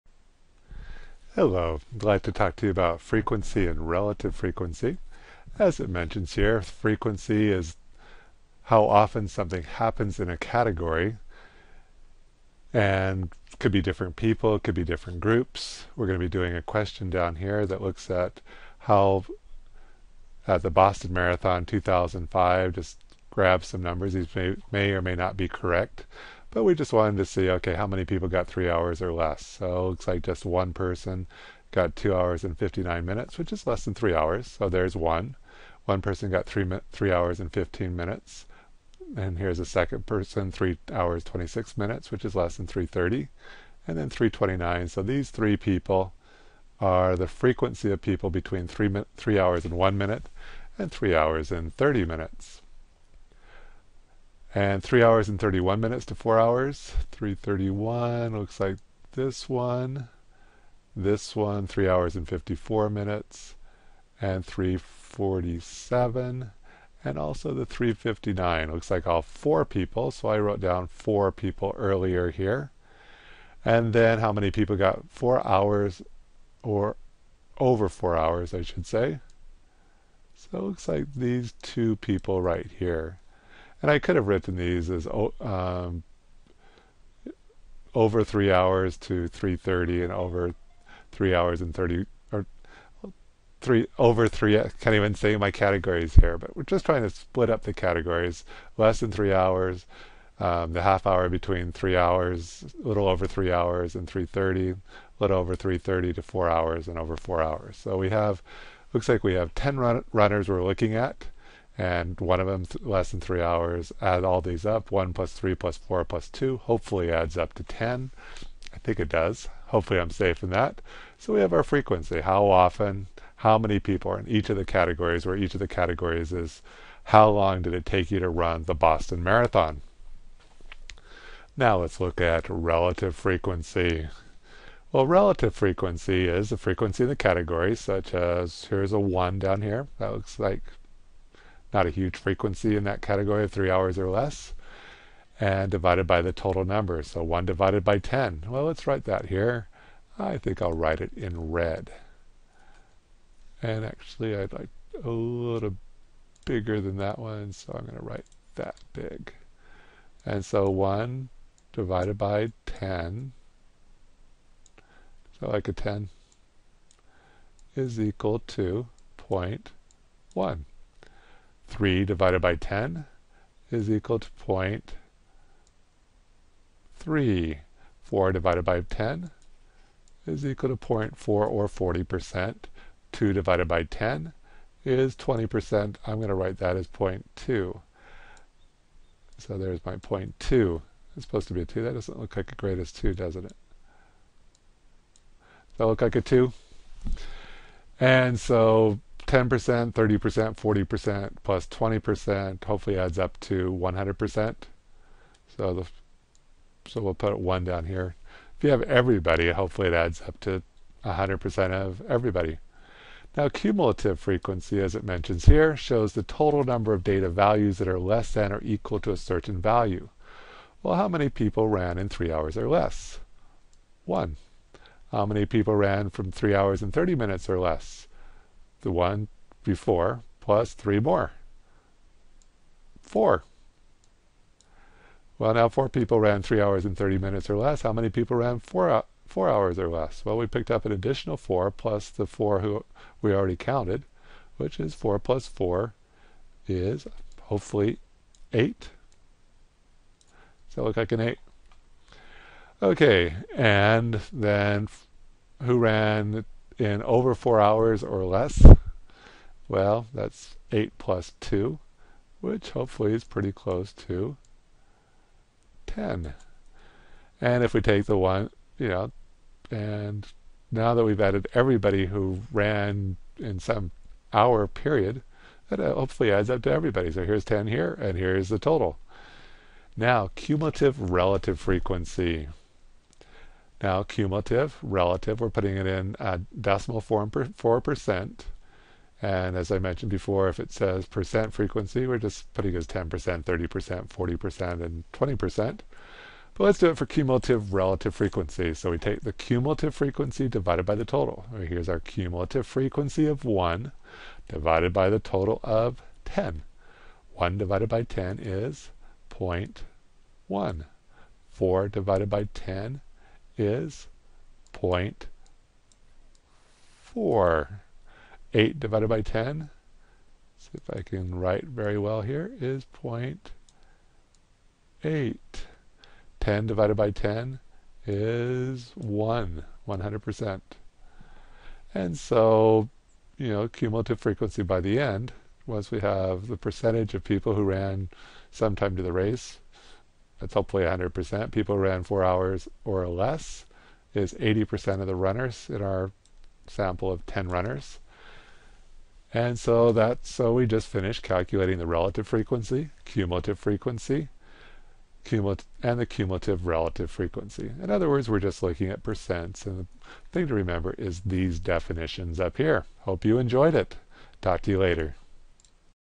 Frequency, Relative Frequency, Cumulative Frequency
This video shows how to calculate frequency, relative frequency, cumulative frequency and cumulative relative frequency